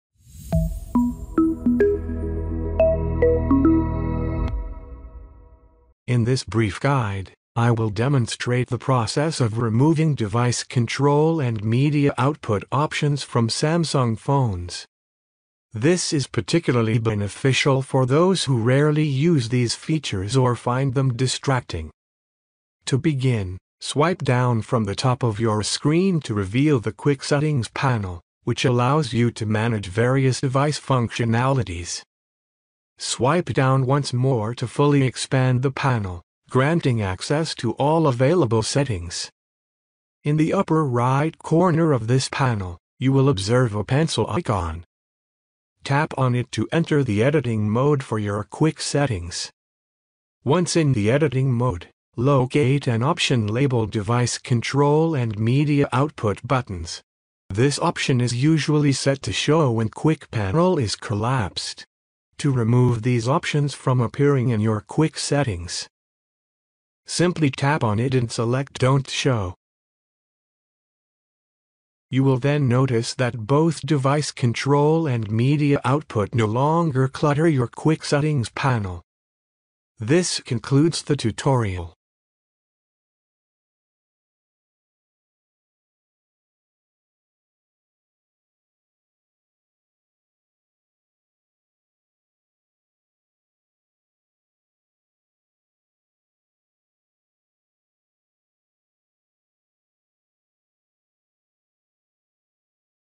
How To Remove Device Control And Media Output In Samsung Phone
In this tutotrial i will show you how you can remove device control and media output in samsung phone.